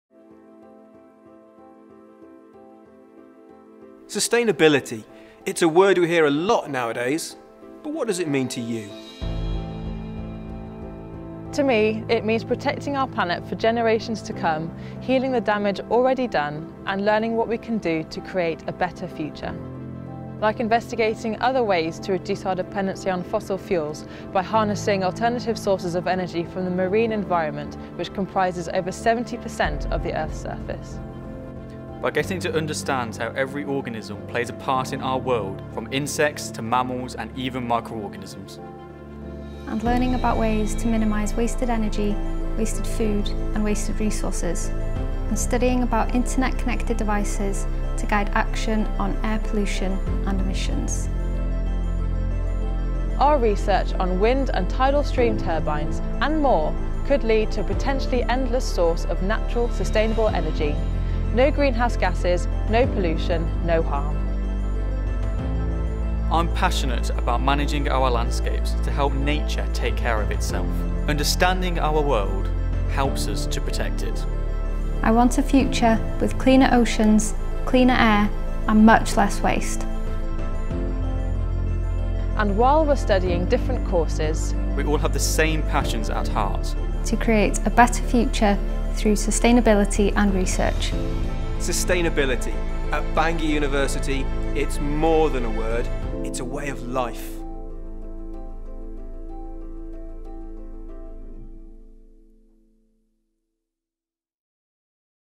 Sustainability at Bangor University
Sustainability - what does it mean to you? At Bangor University, Sustainability isn't just a word - its a way of life. At Bangor University you can be part of the solution to climate change and saving out planet.